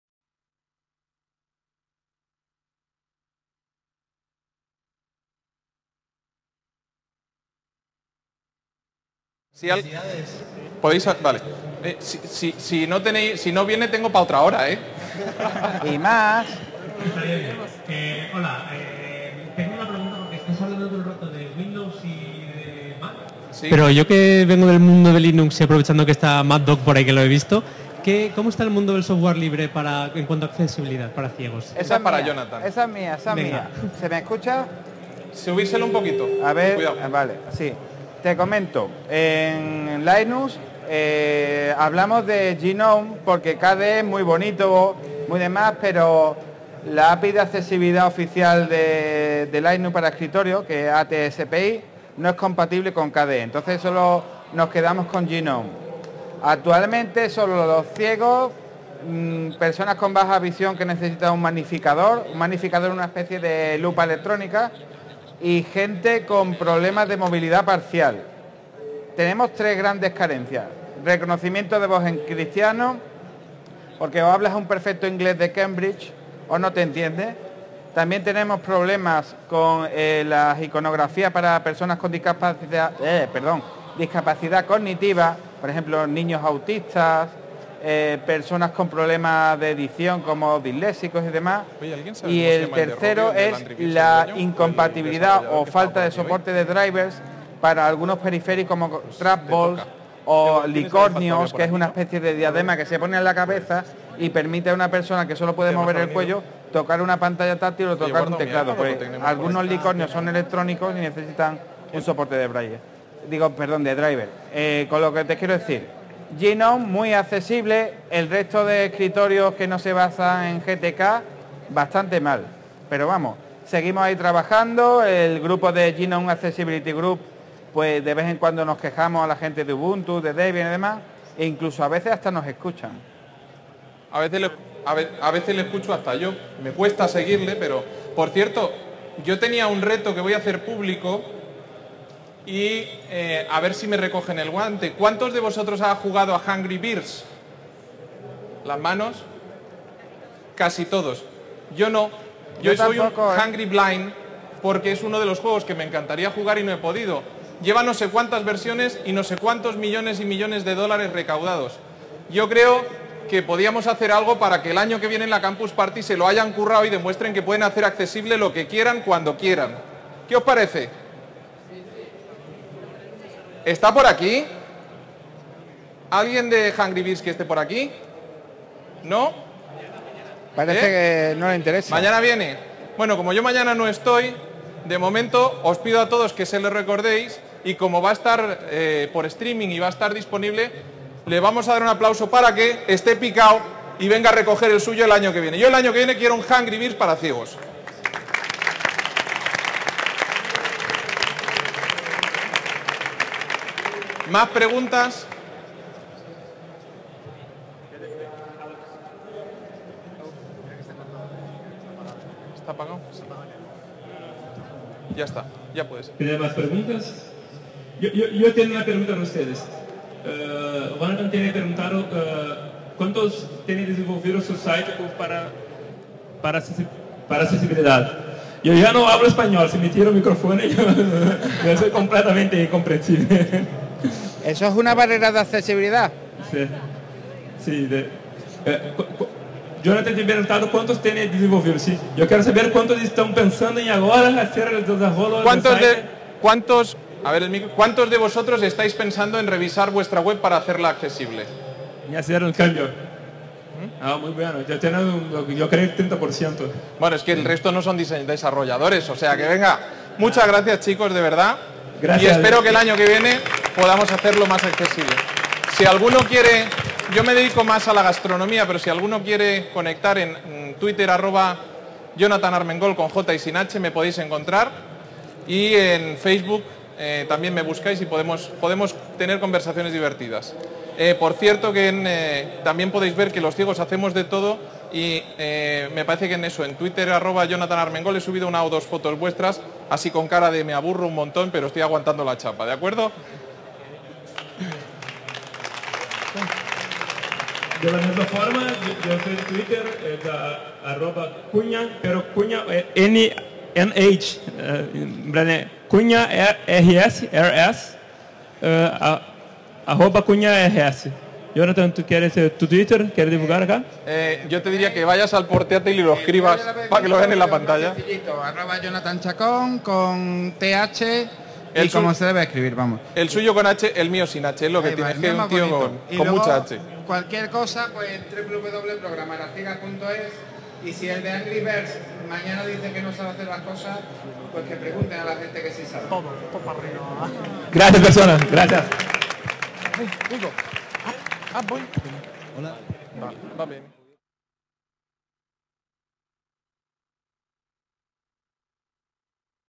CPES15 - Programación para todos (2/2)
Seguro que en Google encuentras miles de alusiones a la importancia de la usabilidad y accesibilidad en Internet. En esta actividad, nuestros ponentes te cuentan su experiencia. No sólo como usuarios avanzados sino cómo profesionales de las TIC y desarrolladores ciegos se enfrentan al reto de crear software, páginas web... No te pierdas esta interesante propuesta en la que no sólo hablarán de accesibilidad sino que te la demostrarán.

Ponentes: Jonathan Armengol. Periodista e instructor en tecnologías para ciegos, se relaciona con la informática desde los tres puntos de vista, usuario, programador y formador. Su postura, es que la accesibilidad para todos tendría que ser una obligación real además de una postura moral. En la actualidad, podríamos decir que si no se crean sistemas accesibles es por desidia o desconocimiento pero en ningún caso por falta de herramientas.
Jonathan Chacón. Consultor en accesibilidad, usabilidad y nuevas tecnologías. Creador del sello Tyflos Accesible Software. Chacón cree en el diseño para todos representado en una frase que siempre usa en sus charlas: la accesibilidad debe ser una base del proyecto no un parche del producto. Ha debutado en la App Store con una versión accesible del buscaminas.
Luis Canabarro Cunha. Coordinador de Relaciones Institucionales de la Prefeitura de Porto Alegre fue coordinador del proyecto @lis-Cibernarium en Porto Alegre y puso en marcha un curso sin precedentes para las personas ciegas - Espacio Braille. Fue elegido como representante de América Latina por el  proyecto Cibernariums en el III Encuentro Iberoamericano de Ministros de la Unión Europea celebrado en la Cumbre de Lisboa.